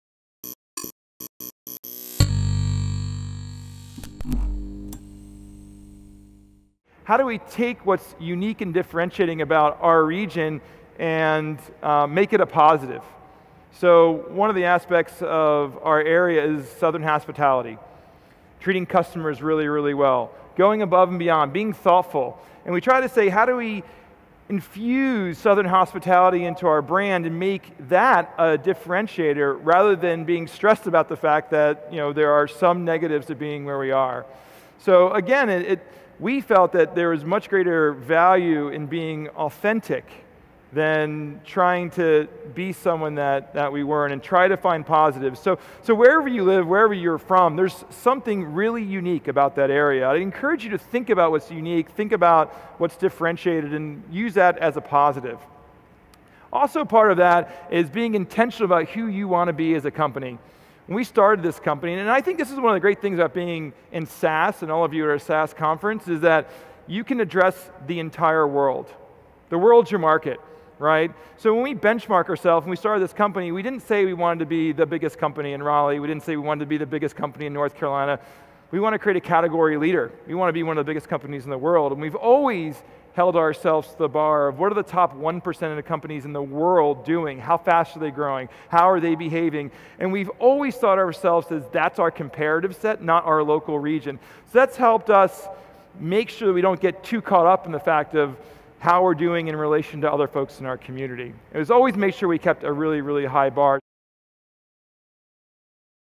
Setting Expectations for SaaS Companies Based Outside the Bay Area with Pendo
Pendo.io is based in Raleigh, NC. As a prenicorn located outside of the Bay Area, Pendo has faced unique challenges. However, some of those challenges apply to all SaaS companies outside of the Bay Area. Like acquiring talent, setting expectations and how to use your location as a competitive advantage. Pendo CEO, Todd Olson, talks about how he sets expectations for Pendo at the global level instead of a regional level and why he encourages other SaaS companies outside the Bay Area should do the same.

Want to join the SaaStr community?  We're the 🌎largest community for B2B software.